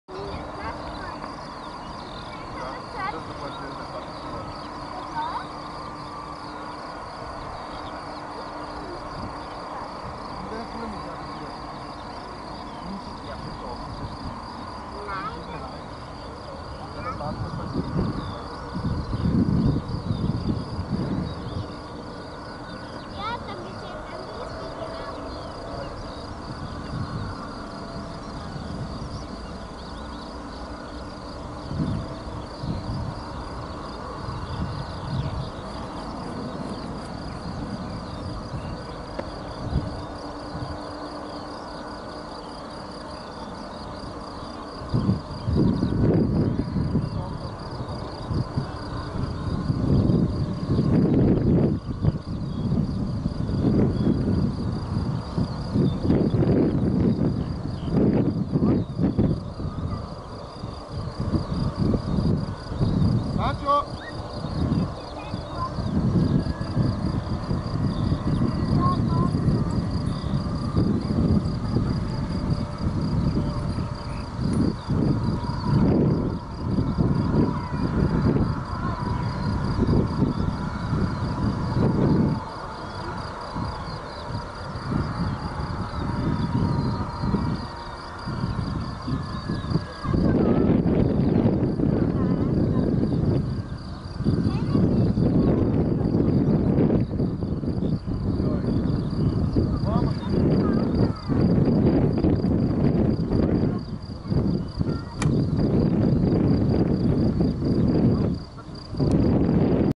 Boeing 777 - HL8252 - Korean Airlines - VIE - LOWW - Wien Schwechat
Boeing 777F (6/2012), HL8252
Korean Air Lines
engine: 2x GE90
sn 37638
Vienna Airport VIE